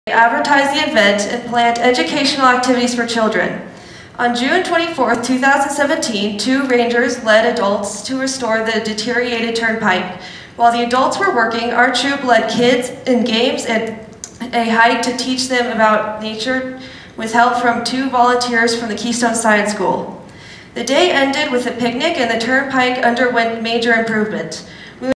Silver Award Girl Scout
This Silver Award Girl Scout described her project at GSCO's Highest Awards celebration at the Silverthorne Pavilion in Silverthorne on May 11, 2018.